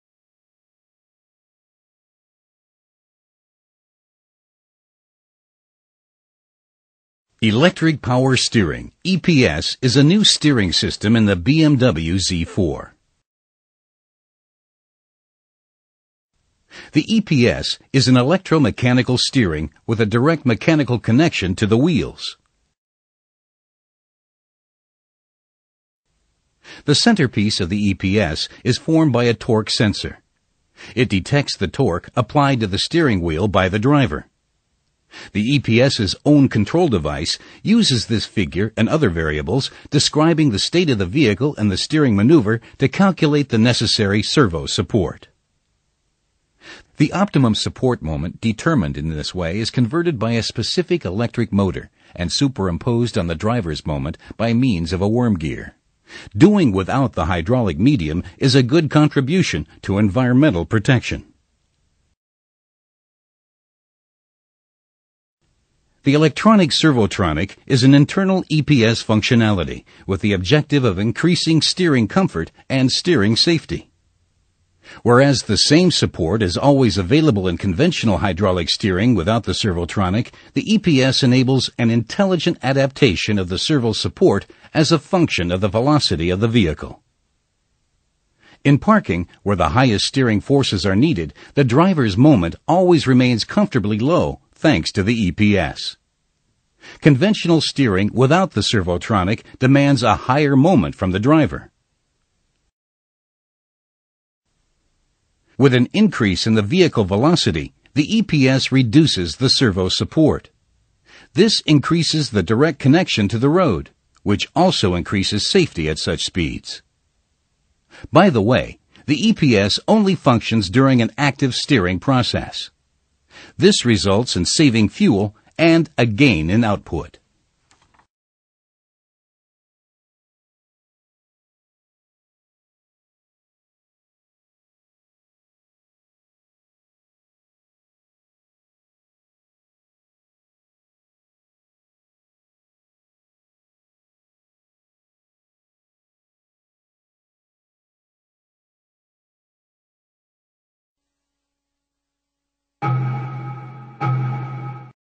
BMW Electric power steering EPS 電能輔助動力方向盤
The electric power steering system makes driving your BMW vehicle even more of a dynamic, responsive experience. The actual level of force applied in turning the steering wheel is precisely measured, and the steering behaviour of the vehicle instantly adjusted to suit the situation. This ensures you enjoy more direct steering. The high-performance Sports mode takes this experience to an even higher level.

Although hydraulic steering systems are used very often in automobiles, electric power steering is becoming more and more widespread. This system offers the benefits of greater fuel efficiency, thanks to its ability to supply its own energy needs according to the current driving conditions, and less tyre wear. In addition, the system requires less space than an hydraulic system, and is remarkably low-maintenance.
Electric power steering delivers a highly flexible, individual steering dynamic. A torque sensor measures the force that the driver is applying to the steering wheel, and adjusts the vehicle's steering characteristics to suit the situation. This ensures a more immediate and direct sports driving experience. A press on the "Sports" mode button enhances this further with an even more sporting steering style.